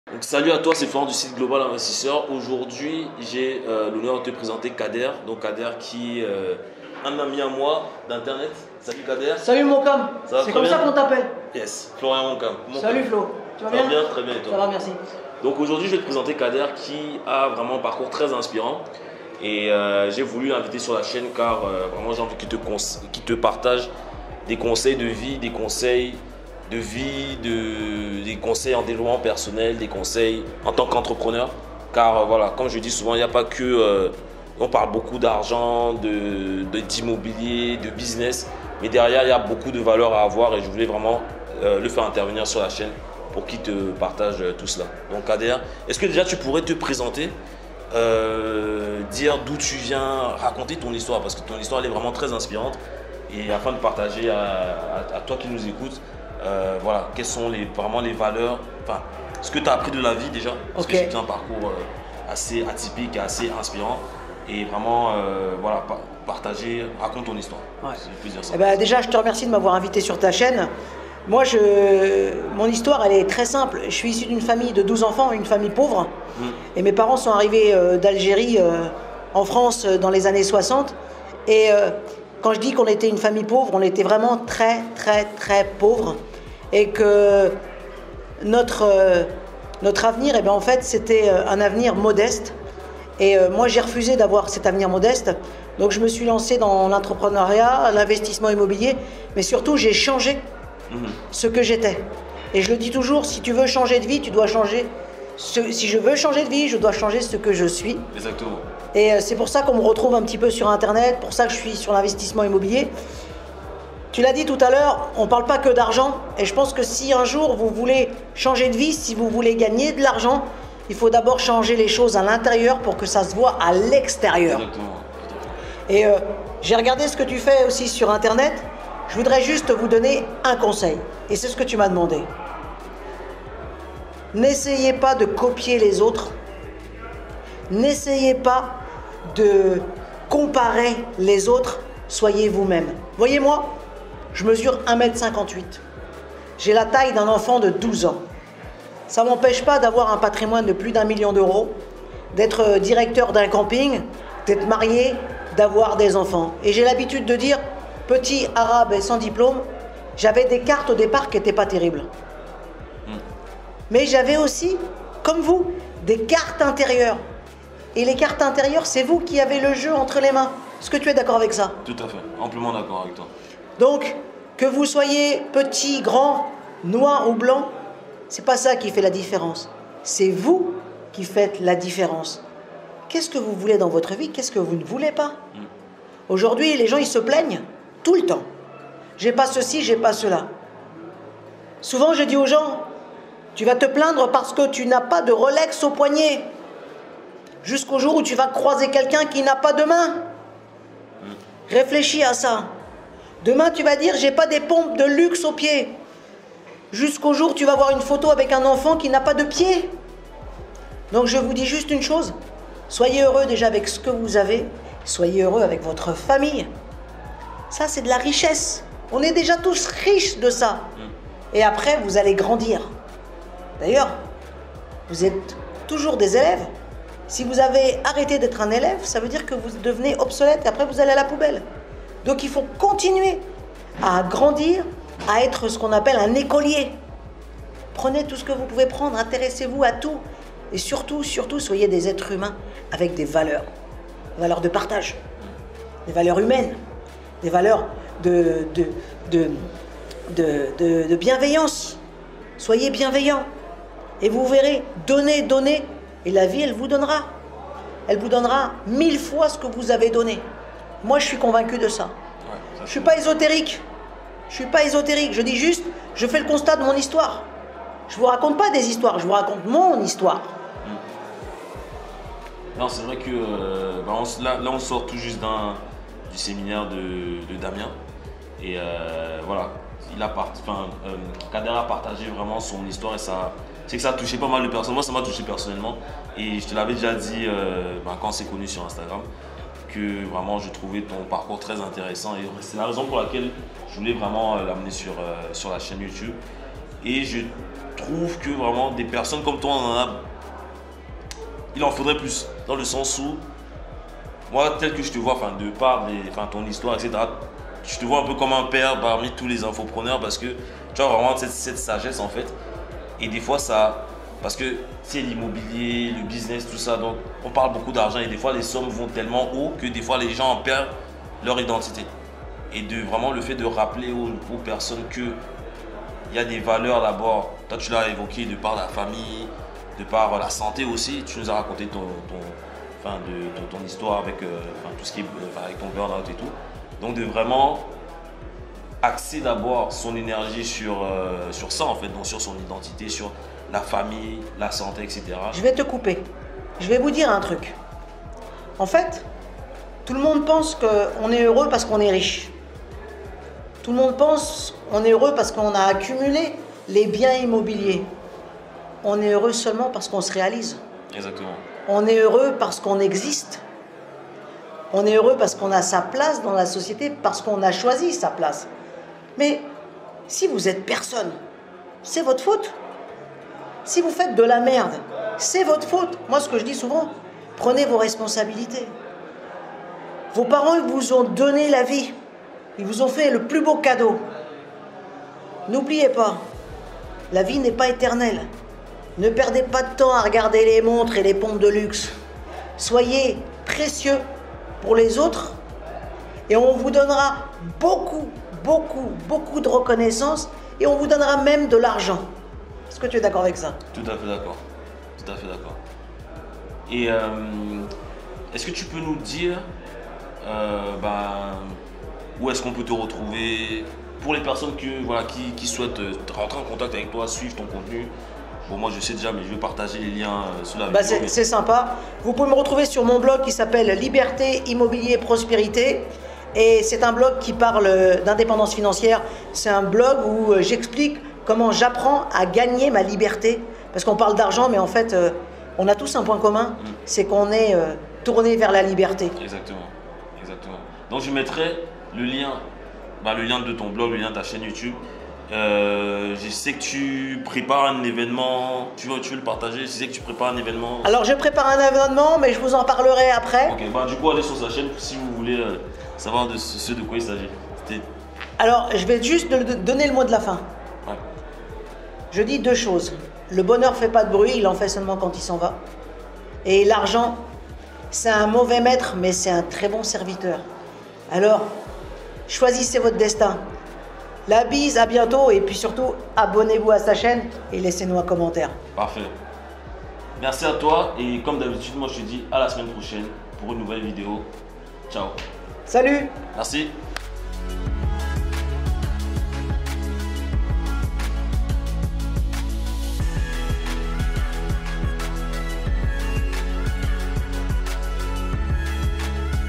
Les Conseils de Vie D'Un Millionnaire Parti De Zéro - Kader
Dans cette nouvelle vidéo je te présente Kader, Investisseur immobilier, qui a un parcours remarquable. A travers cette vidéo Kader te partagera des leçons de vie très fondamentales.

Avertissement